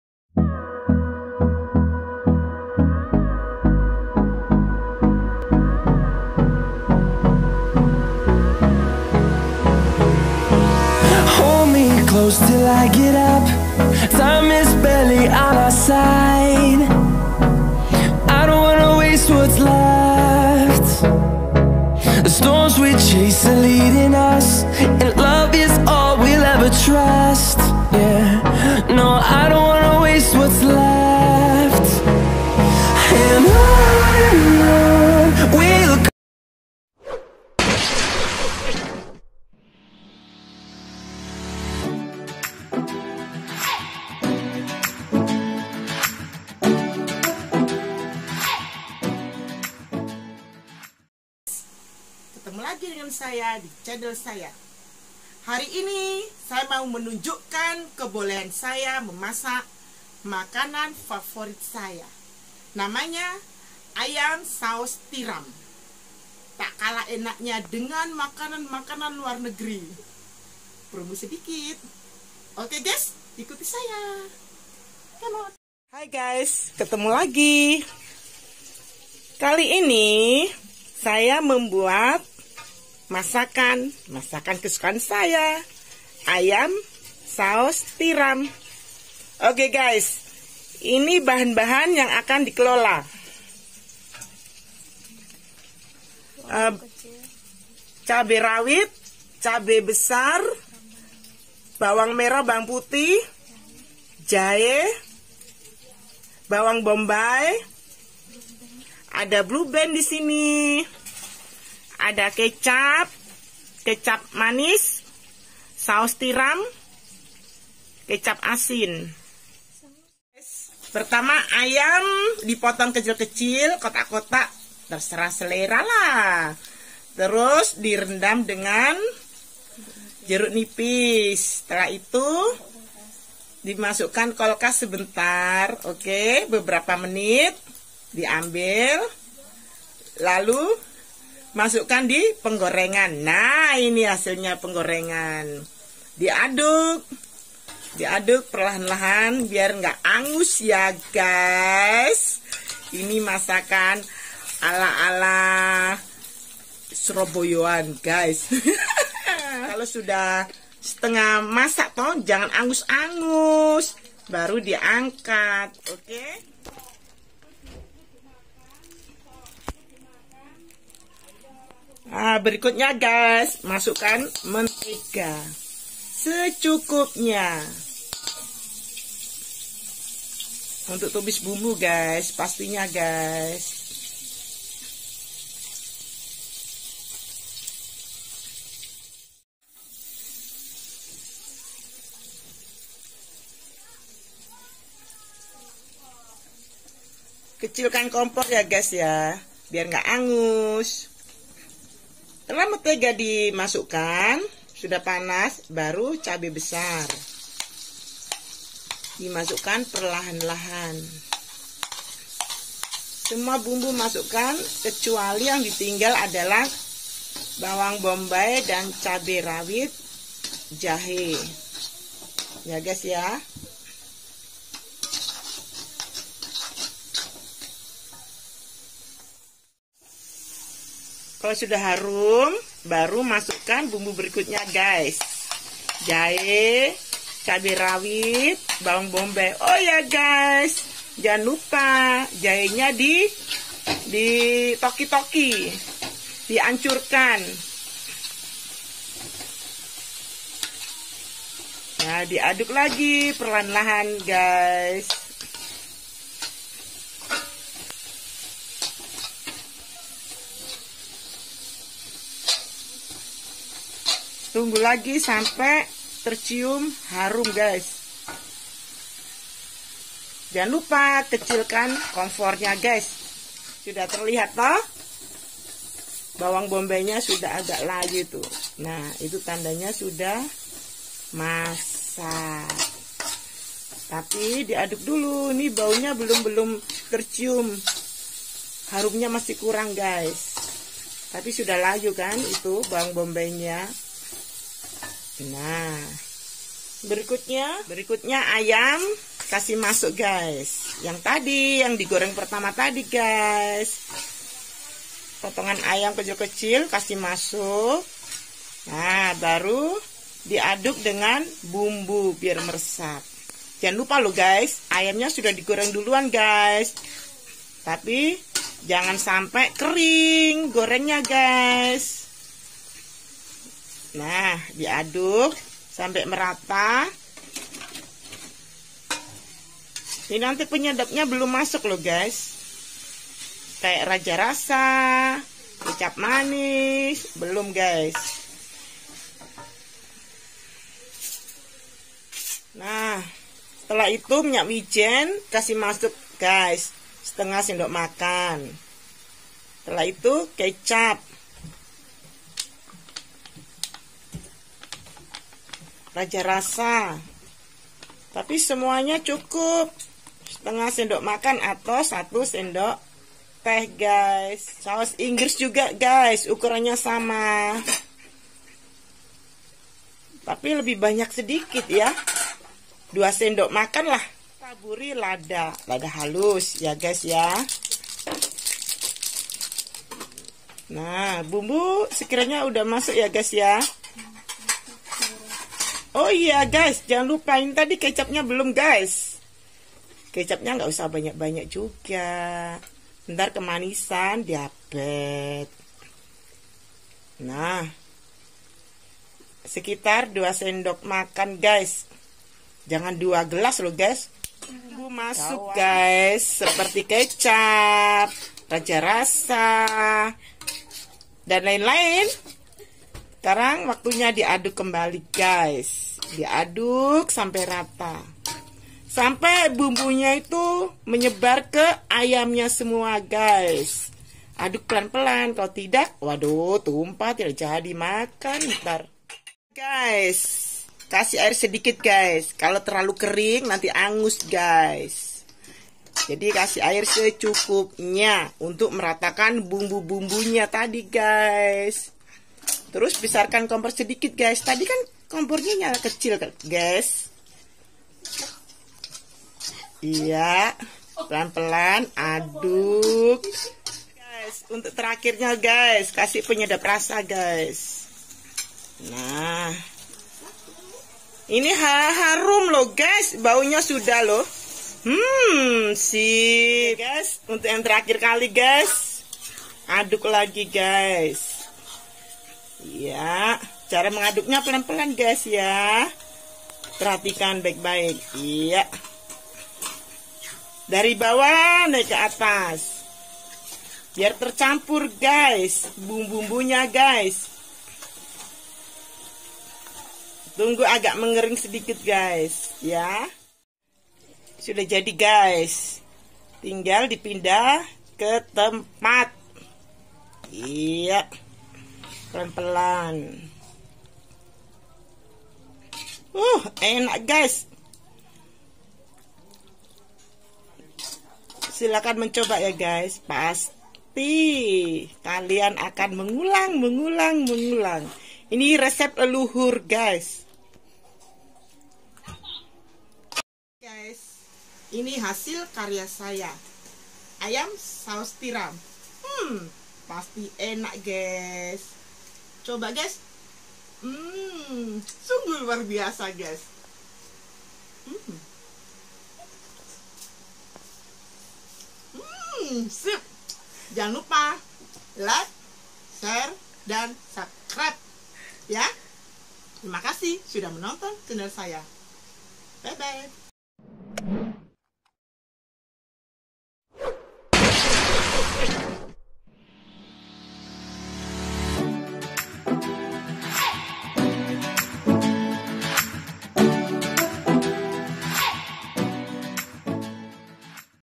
Toturial Ayam Saos Tiram, (menu andalan keluargaku)
menu makanan ayam saos tiram merupakan makanan kesukaan keluarga, rasanya lezat dan murah biayanya utk membuatnya.

Selamat Mencoba
untuk bahan nya :
- 1/2 ayam
- bawang putih 2 siung
- bawang merah 5 biji
- cabe besar 3 biji
- bawang Bombay 1 biji
- cabe kecil ( sesuai selera )
- jahe 1 ruas jari
- saus tiram
- minyak wijen
- kecap asin
- kecap manis
- lada bubuk
- saos tomat
- raja rasa
- saos Inggris
- penyedap rasa secukupnya
- gula dan garam secukupnya
- air jeruk nipis secukupnya
- margarine secukupnya